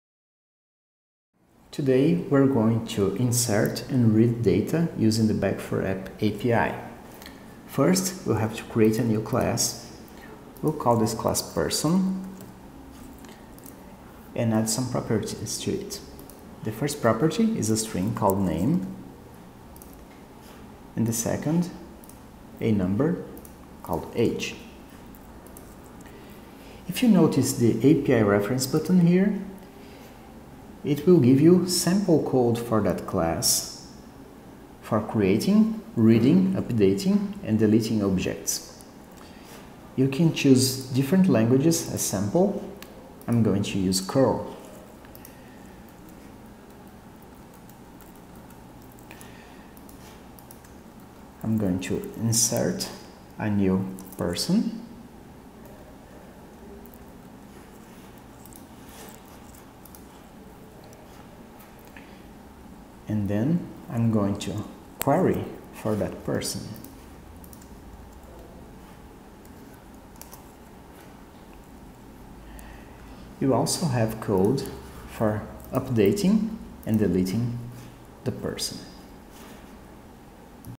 Database Browser
How to save and retrieve data using Back4App cloud database.